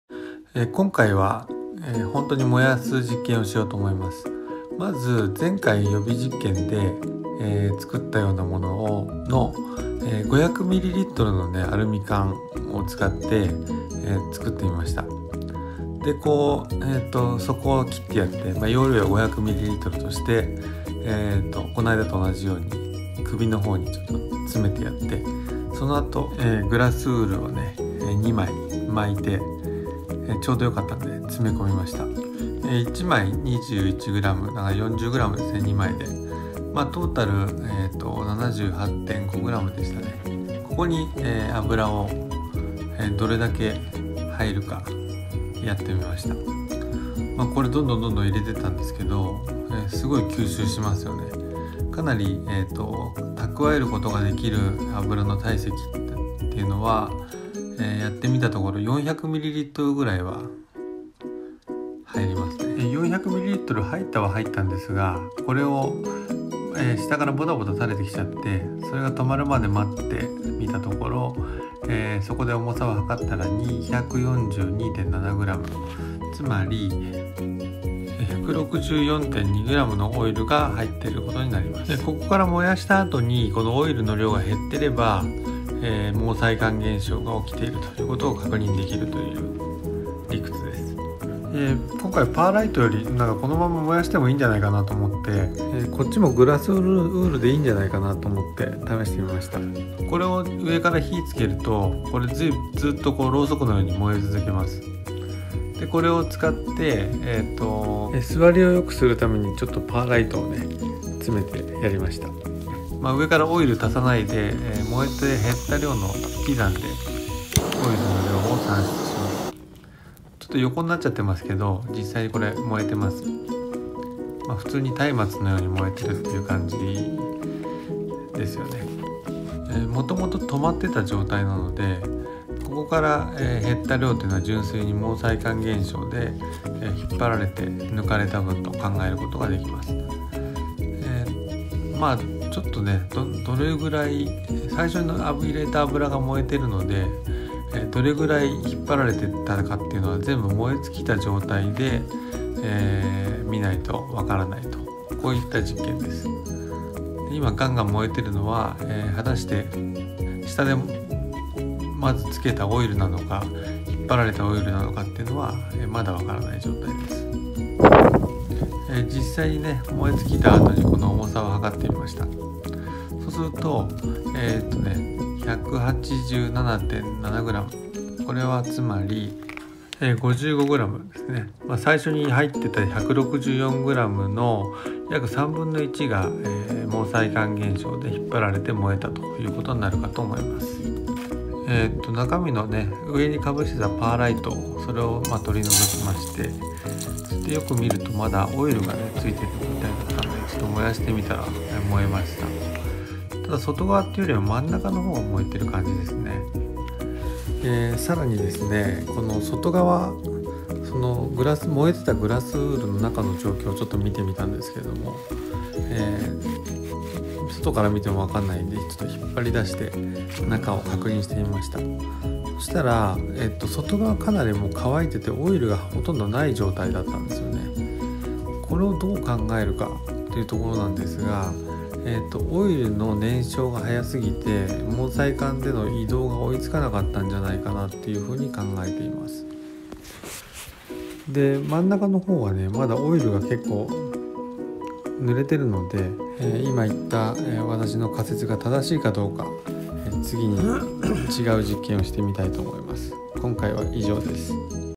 ロケットストーブで廃油を燃やす！燃焼実験 Partー1 毛細管現象の効果 confirmation of capillary effect
燃焼実験始動しました！今回のポイントは、毛細管現象で油が移動するかどうかです。具体的にはグラスウールに油を飽和させておき(ポタポタ落ちない状態)、燃焼箇所に付けて燃やした時に、そこから油が供給されるかを評価しました。
The purpose of this experiment is to confirm the capillary effect during combustion.
Procedure is as follows,
1) saturate an oil into the grass wool can
2) put the can on the burning spot
    (and watch if it provides the oil by capillary effect or not)
3) confirm the amount of decreased oil of the can